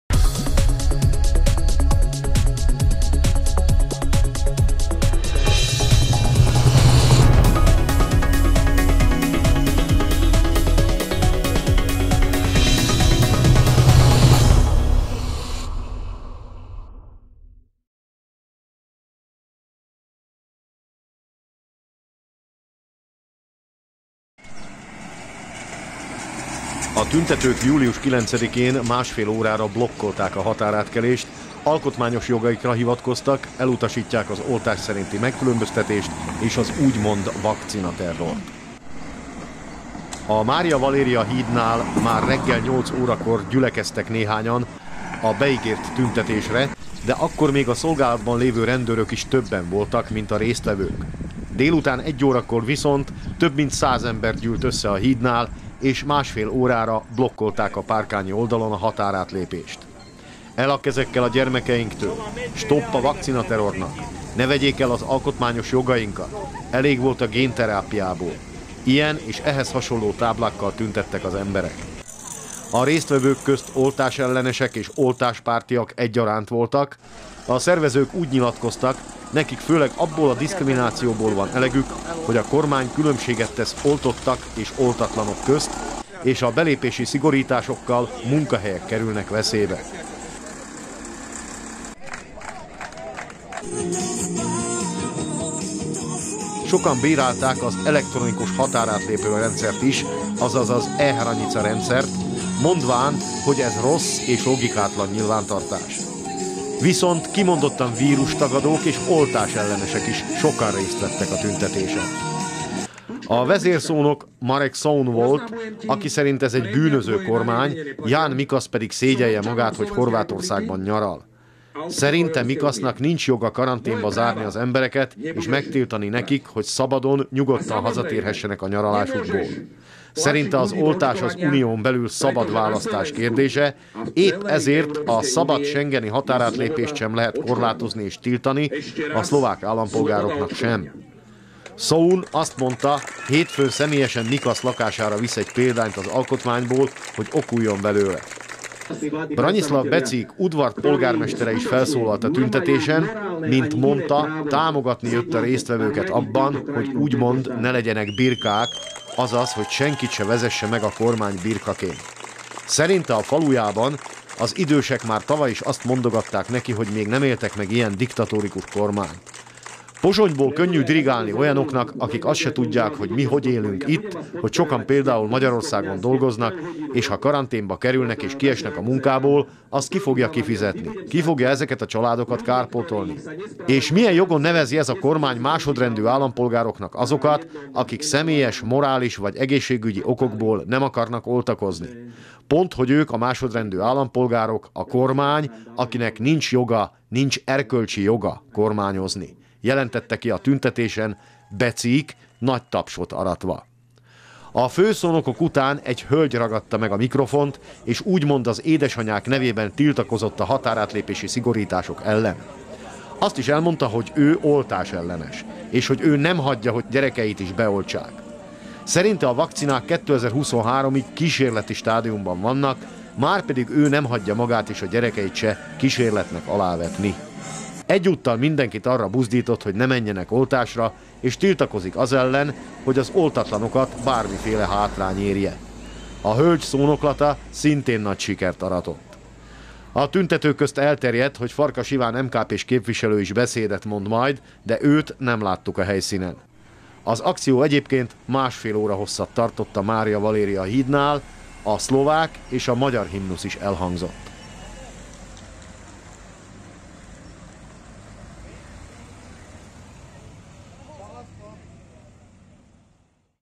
SPRÁVY - HÍREK - 10072021 - FHD
V  Štúrove sa protestovalo aj  proti „vakcinačnému teroru“
A„vakcinaterror” ellen is tiltakoztak Párkányban